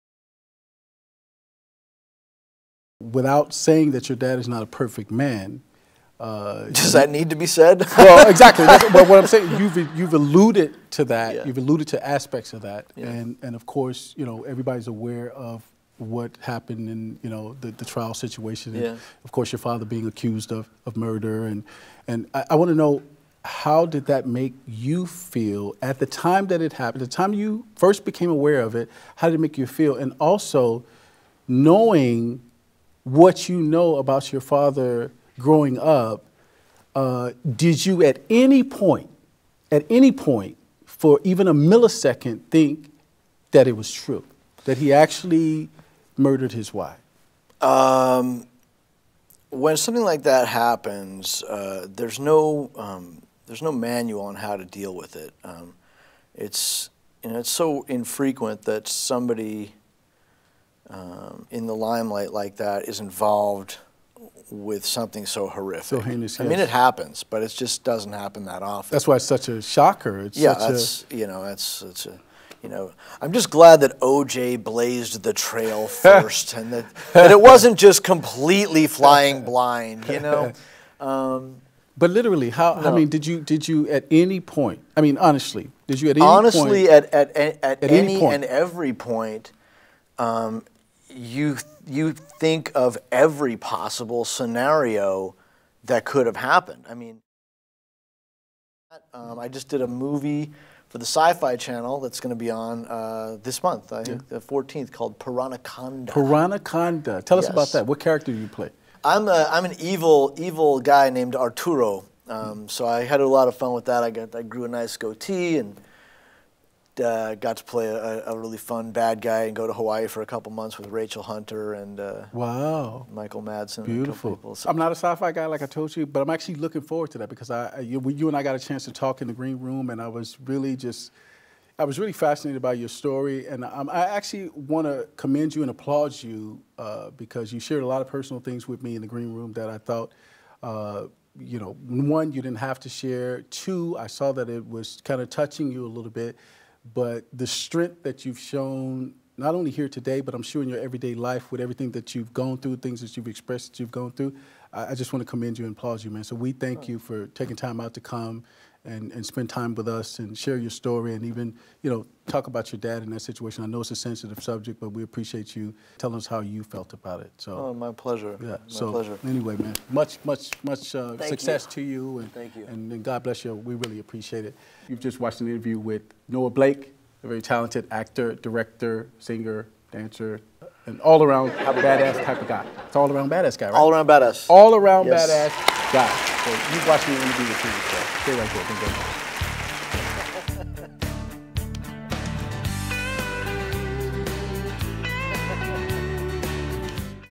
Actor Noah Blake's shocking comments about his dad
Noah Blake shares his thoughts about his dad, actor Robert Blake, having been formerly accused of murdering his wife Bonnie Lee Bakley. He also discusses his new role in the Sci-Fi television show Piranhaconda.
The InterView with Terrance Bell (A controversial new talk show featuring celebrity interviews and everyday people with extraordinary talents and stories). Premiering soon.